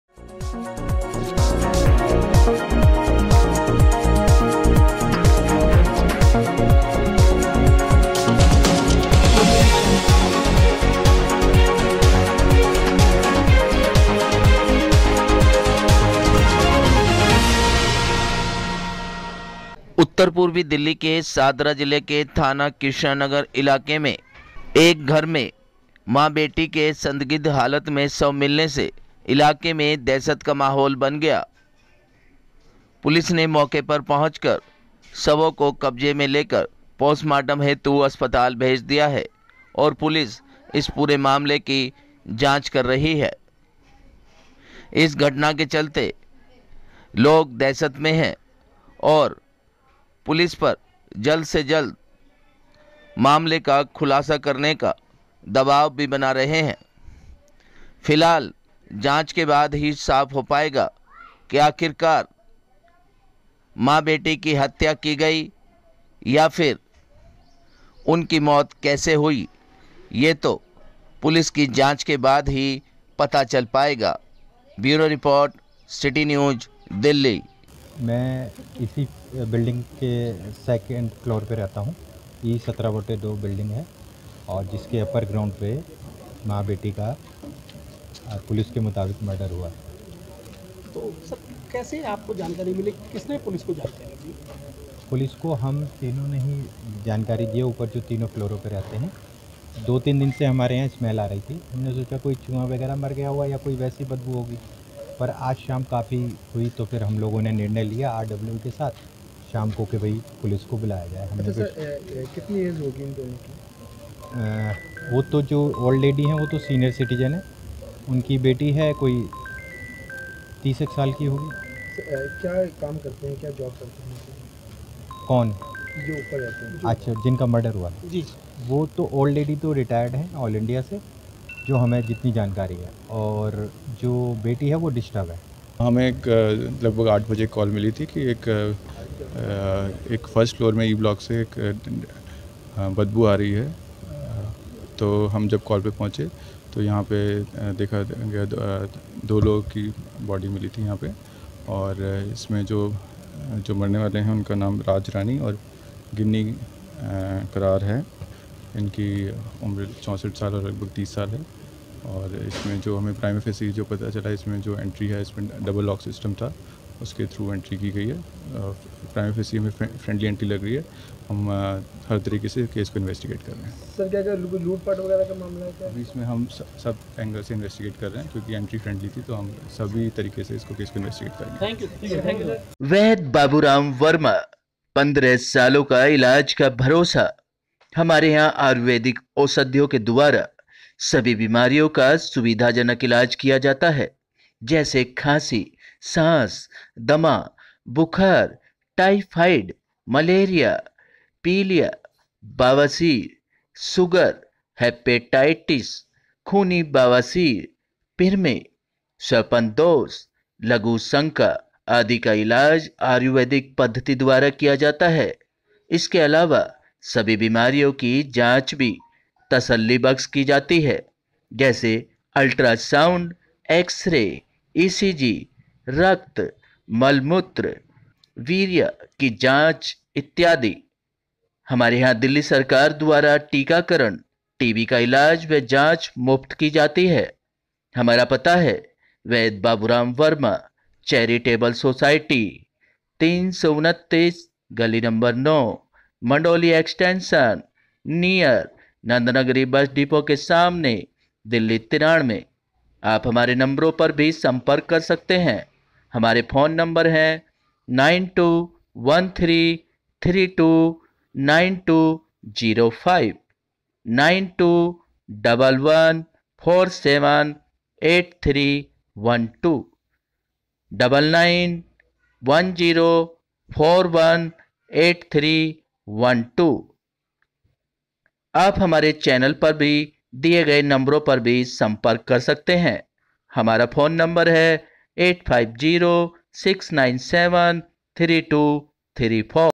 CITY NEWS DELHI - कृष्णा नगर इलाके में मां बेटी की संदिग्ध हालत में हुई मौत ।
थाना कृष्णा नगर इलाके में मां बेटी की संदिग्ध हालत में हुई मौत । पुलिस मामले की जांच में जुटी । इलाके में दहशत का माहौल बना । पुलिस ने शव को कब्जे में लेकर पोस्टमार्टम हेतु अस्पताल भेजा । घर में दुर्गंध आने पर पुलिस को की गई कॉल